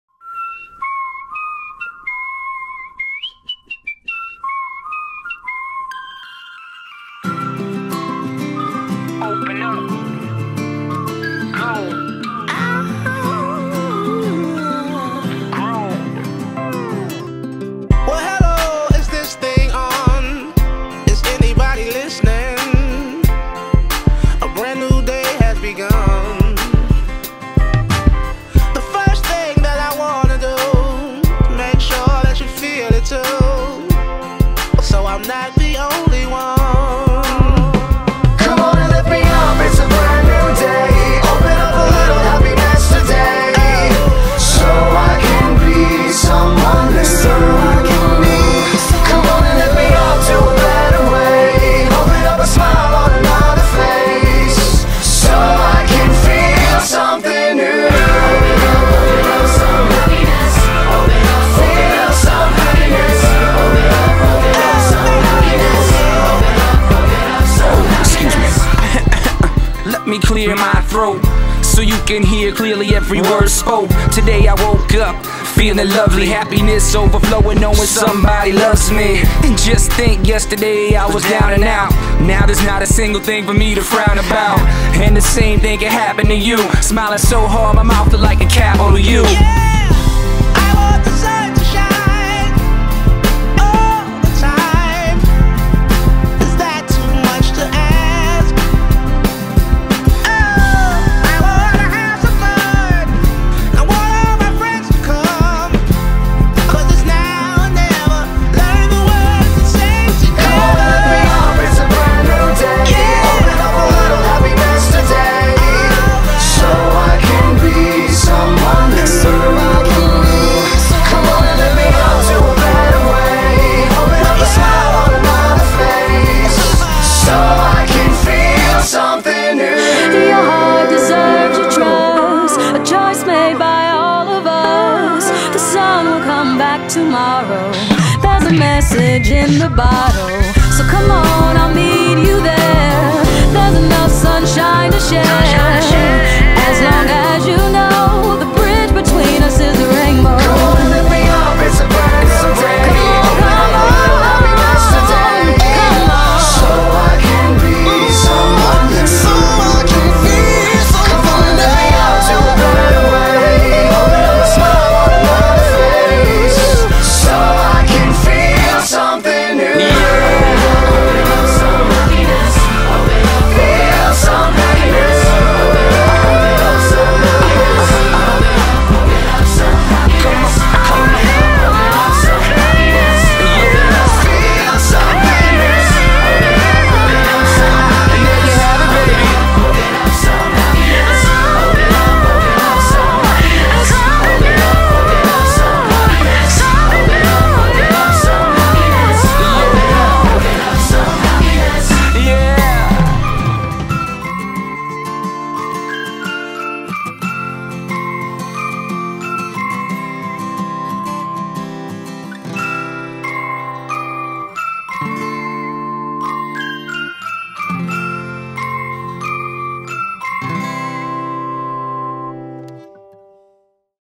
The University of Tennessee Coca-Cola Happiness Vending Machine Highlight Photos
Coca-Cola set up a special vending machine on The University of Tennessee's campus. Check out some photos of the Vols opening up some happiness!